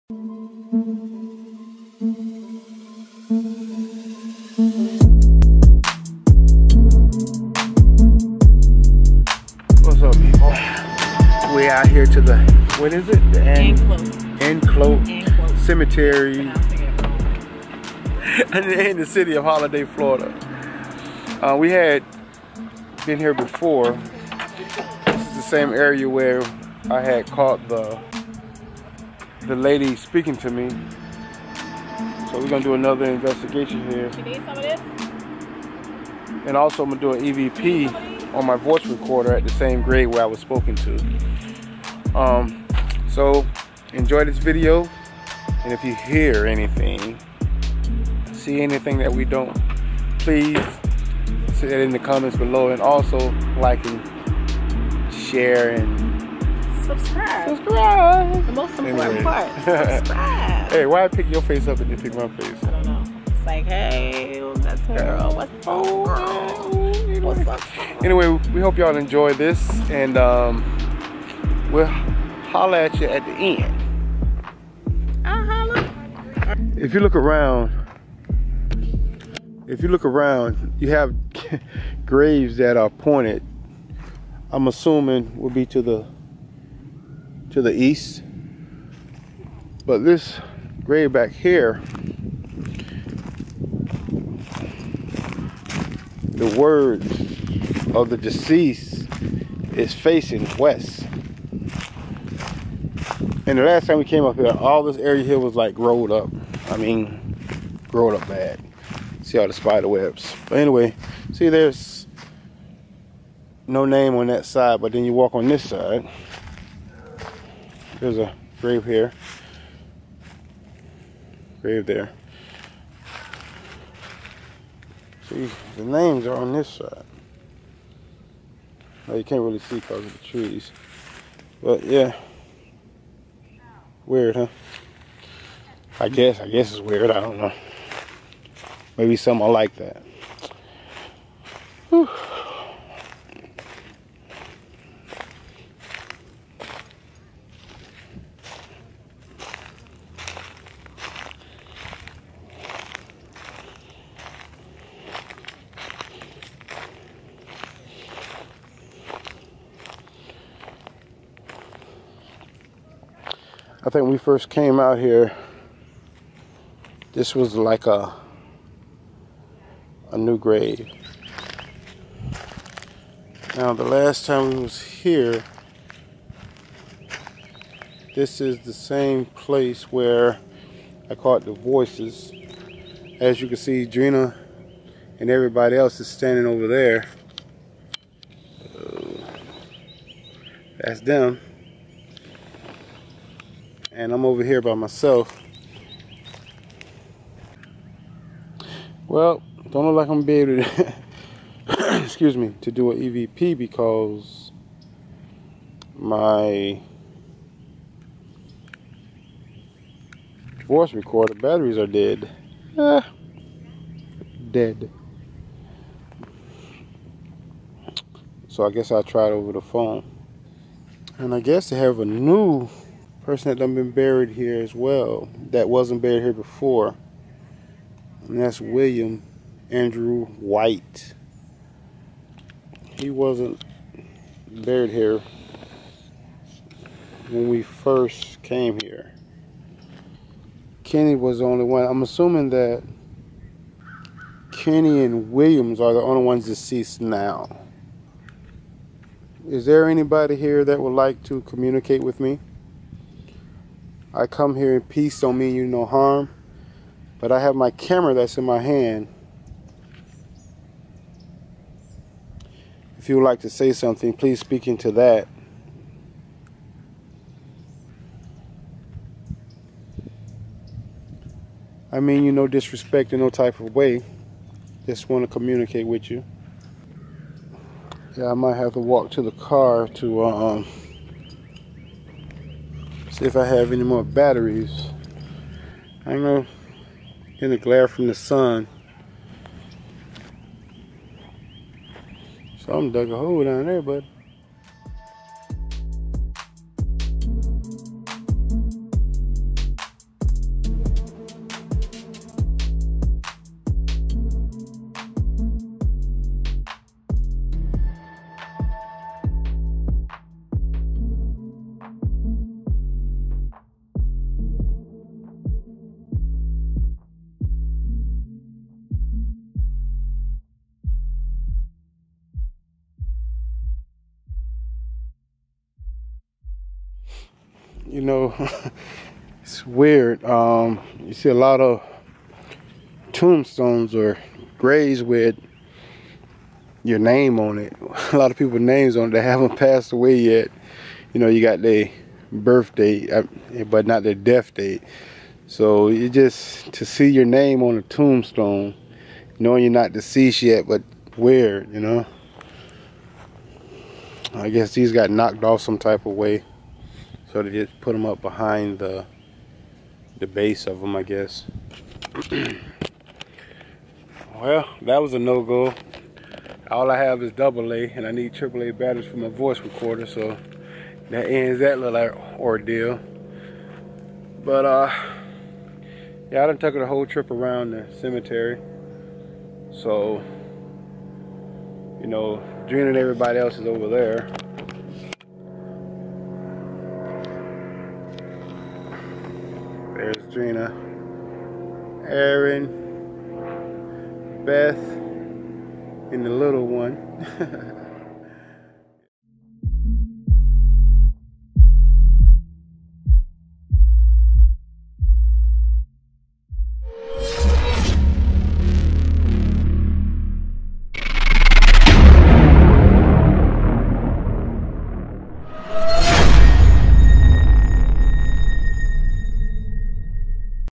Anclote Cemetery - On the Search For More EVP's
We went for a final visit to Anclote Cemetery.  Last  time we were here we heard a clear females voice speak to Mike.  Come see if she will speak to him again.  And we are still trying to find out what he did!!?